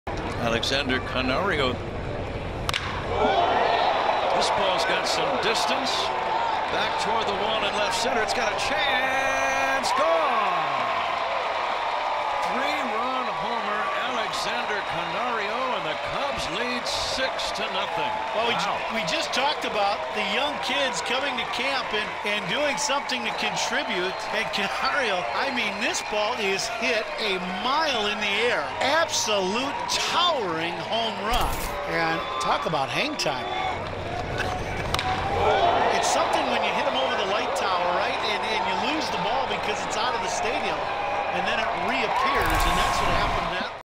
Alexander Canario's 3-Run Home Run
Canario clobbers a 3-run shot to left